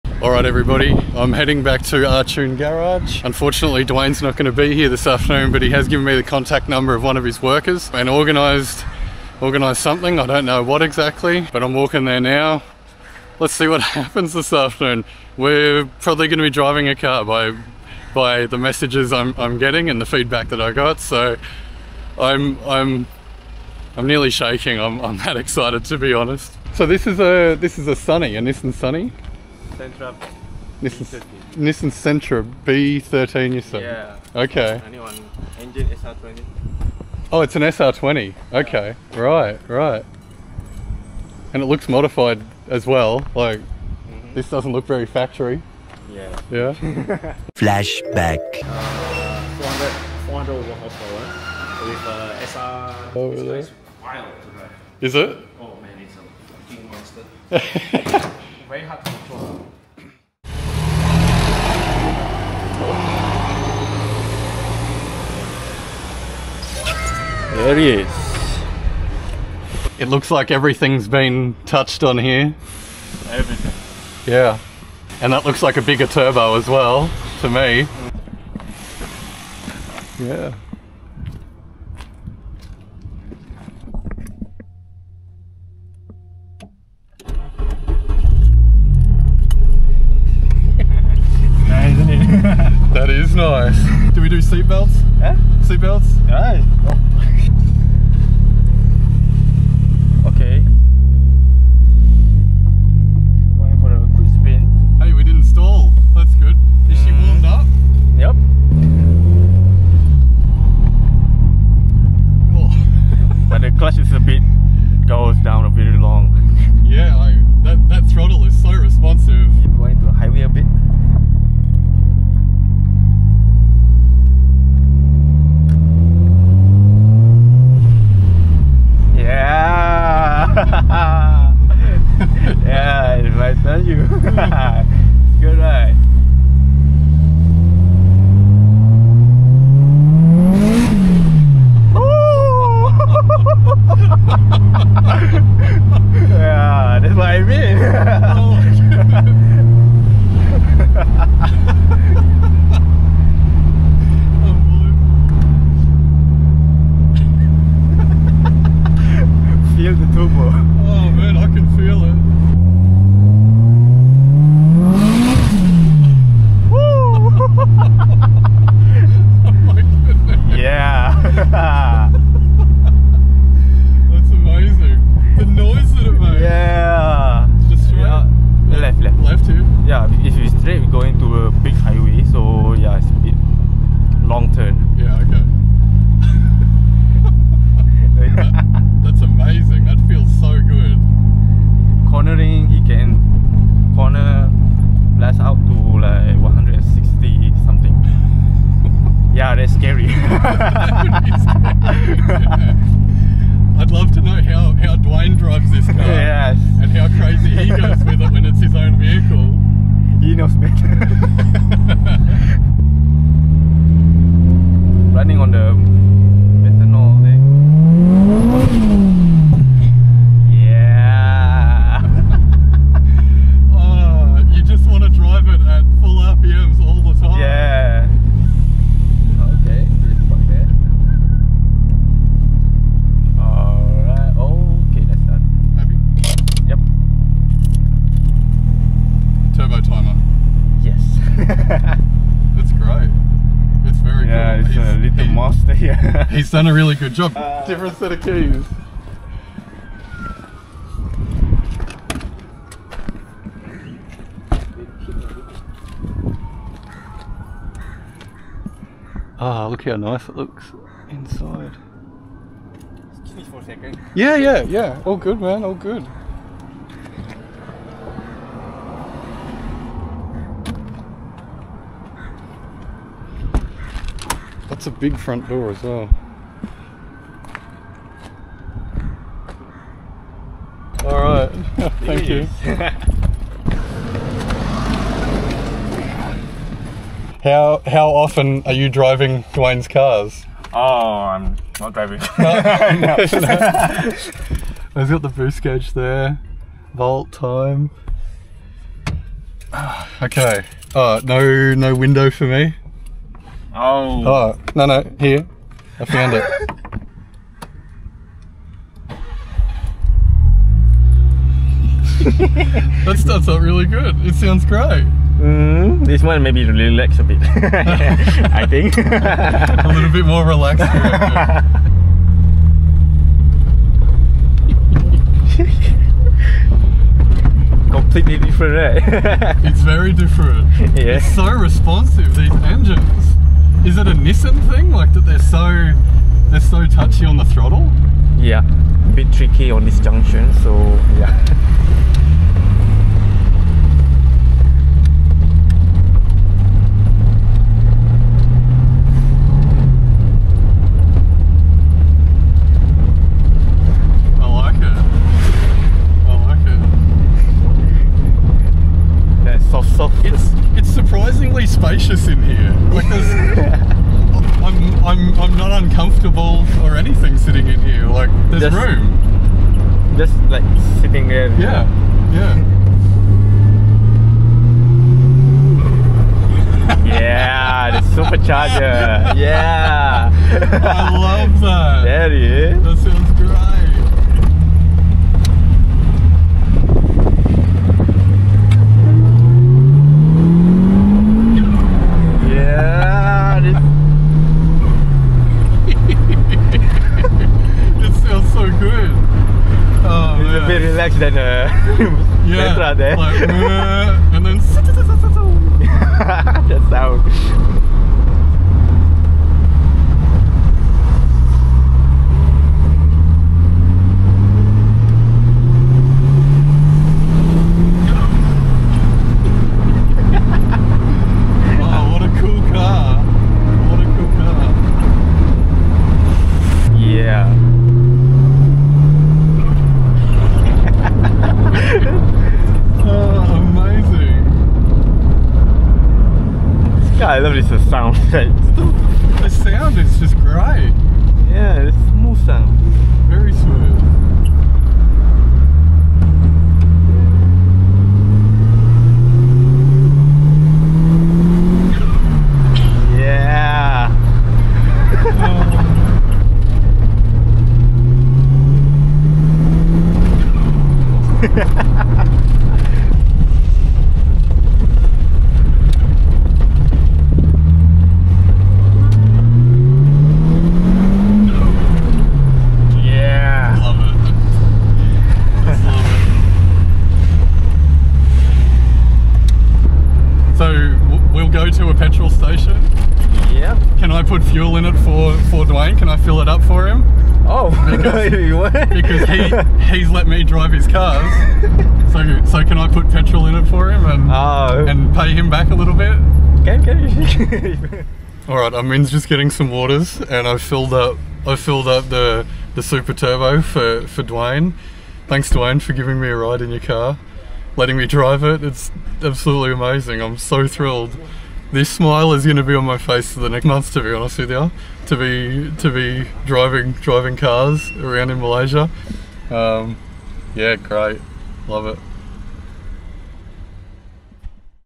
Big turbo SR20 Nissan Sentra & March Super Turbo driving around the streets of Malaysia 🇲🇾
Huge shout-out to Dwayne for letting me drive his cars, thanks! 💪

Experience the raw power and sound of two turbocharged (and one supercharged) beasts tearing up the streets of Johor Bahru, Malaysia! 🏎️💨
In this video, I drive a 400hp Nissan Sentra and the rare Nissan March Super Turbo. Listen to the intoxicating turbo spools and witness the sheer force these modded machines unleash on Malaysian roads.
🔥 Highlights:
Turbo noises
Acceleration
Engine bays and modifications

Whether you're a JDM enthusiast, boost junkie, or just love the thrill of high-performance cars, this video is a must-watch!
👇 Drop a comment telling us which car you'd rather drive!

Timestamps:
0:00 Intro
0:26 Nissan Sentra overview
1:24 Driving the Sentra
4:09 March Turbo overview
5:07 Driving the Super Turbo
8:17 Outro